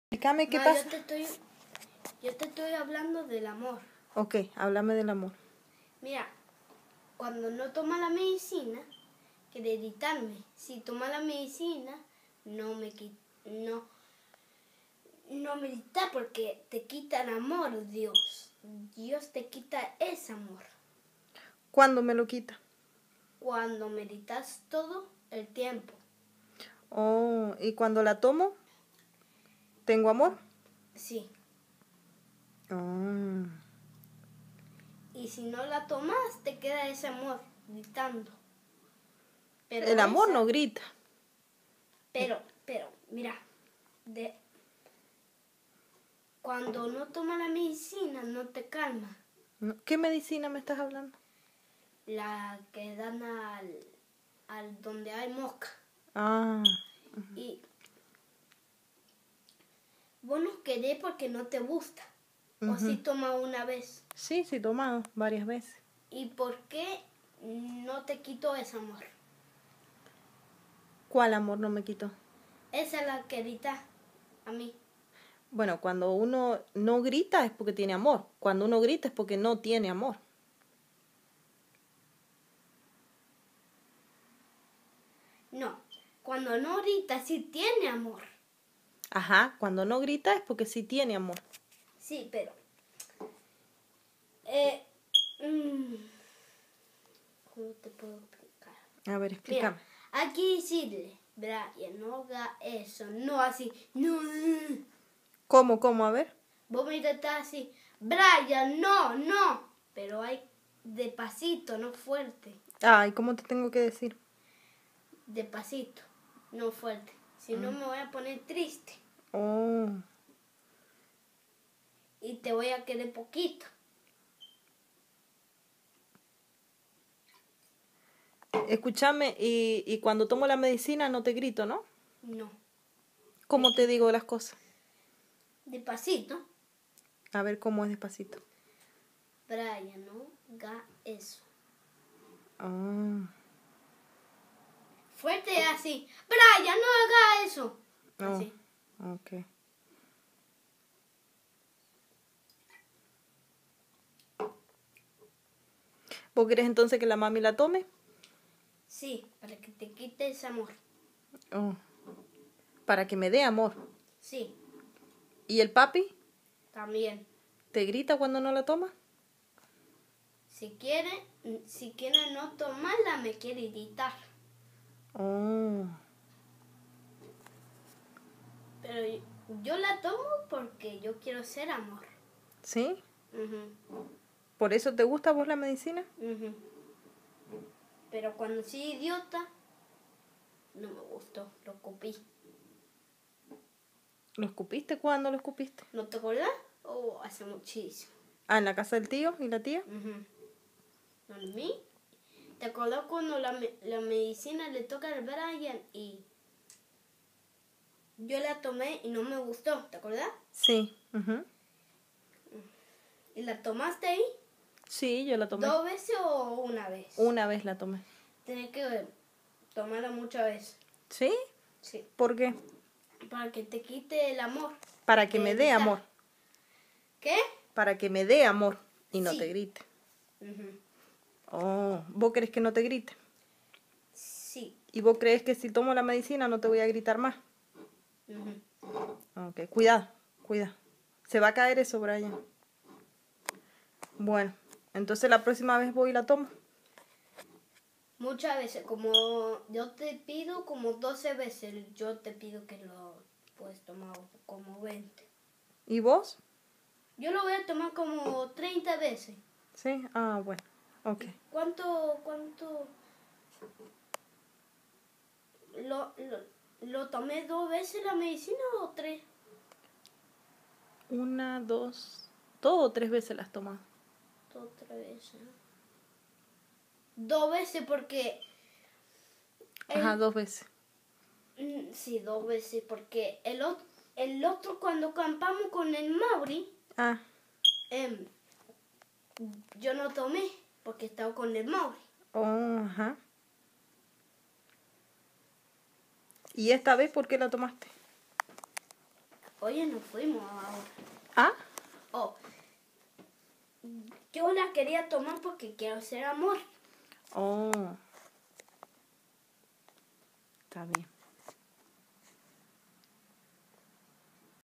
Brian quiere ser amor
Con sólo seis ańos ya sabe lo que quiere en la vida